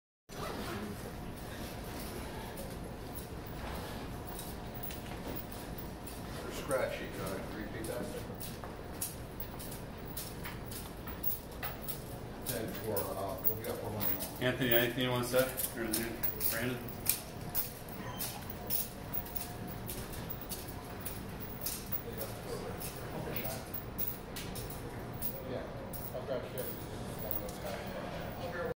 Brandon Gambardella sentenced for fatal shooting
Brandon Gambardella, 20, sentenced to 15-40 years in prison on Feb. 13, 2020, for fatally shooting Anthony Bonney, 17, at a park in Freeland, Pa, on June 18, 2018. Pennsylvania State Police alleged Gambardella chased after and fired rounds at Bonney after Bonney passed off drywall shavings and baking soda as cocaine.